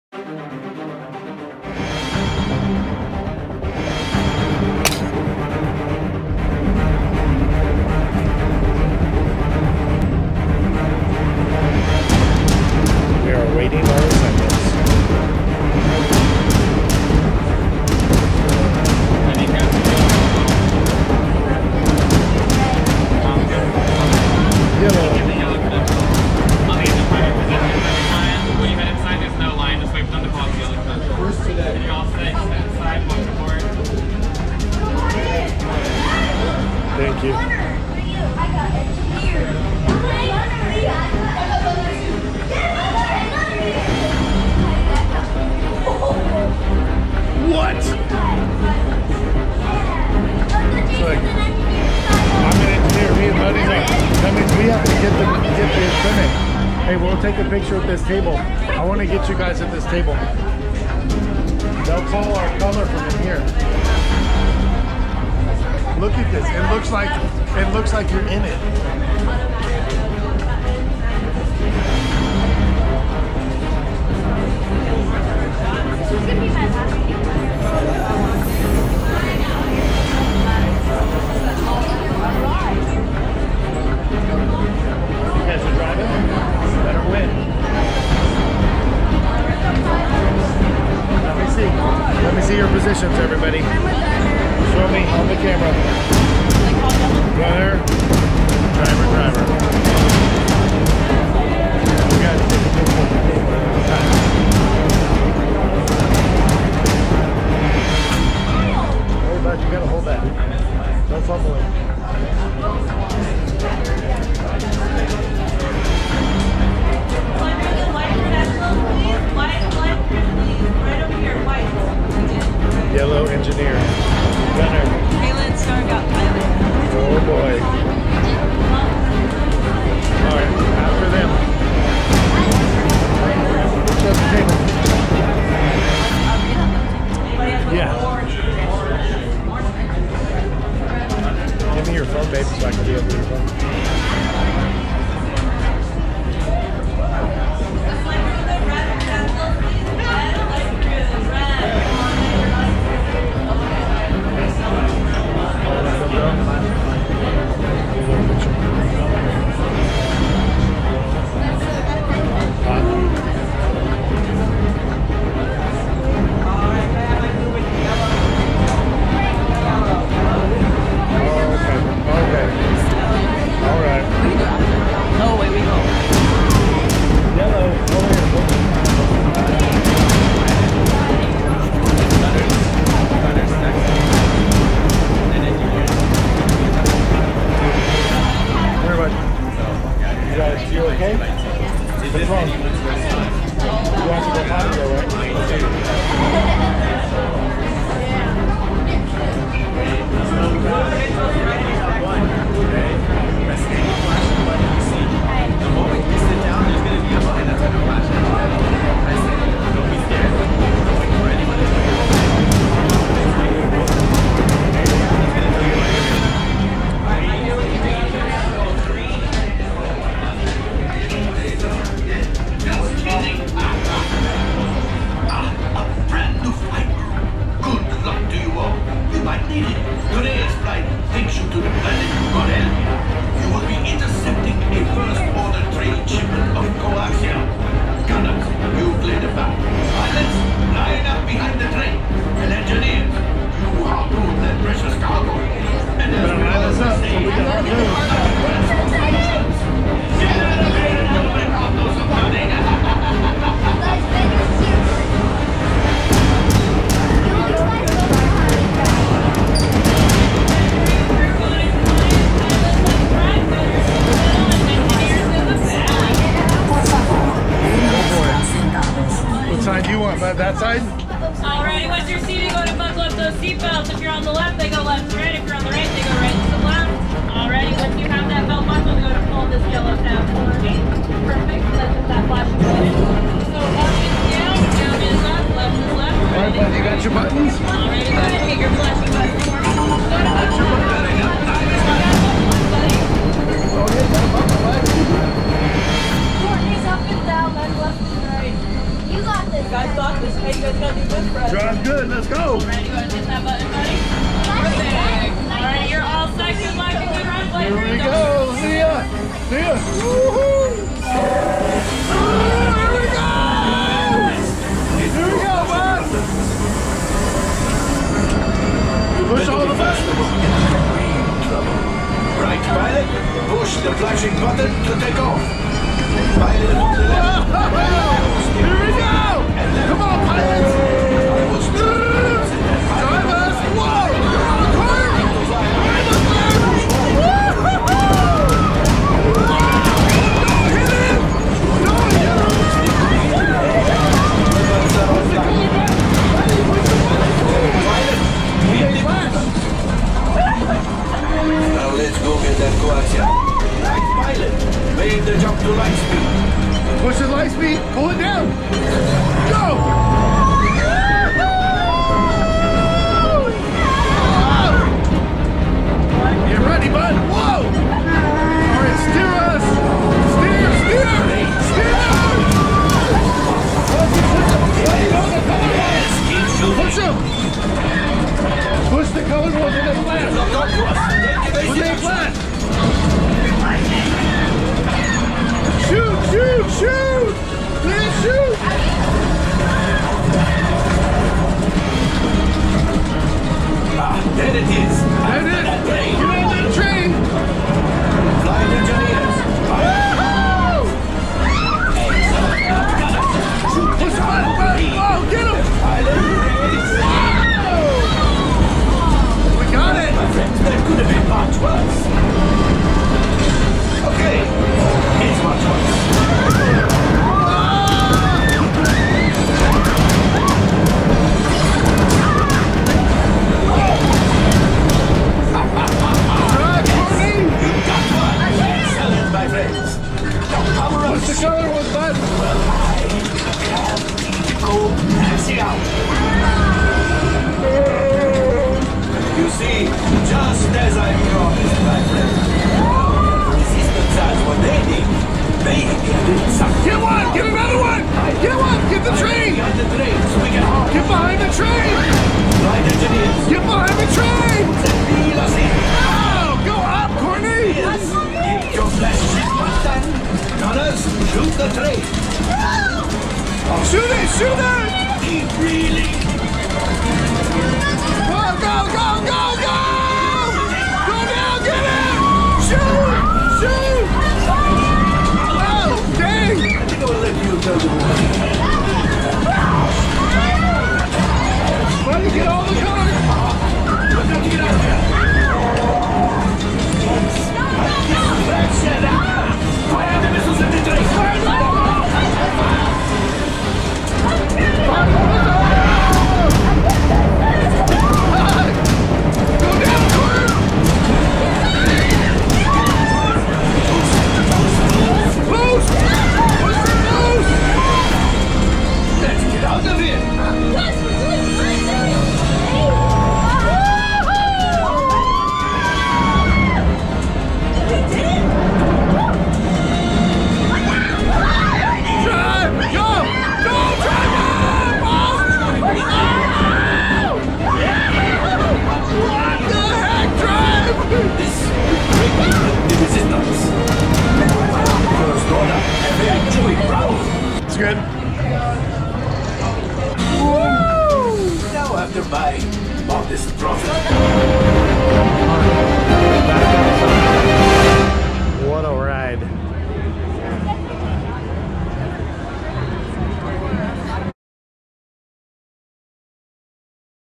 Millennium Falcon - Disneyland Park's Star Wars: Galaxy's Edge
Watch as the entire Family has their first experience on the Millennium Falcon at Disneyland Park's Star Wars: Galaxy's Edge!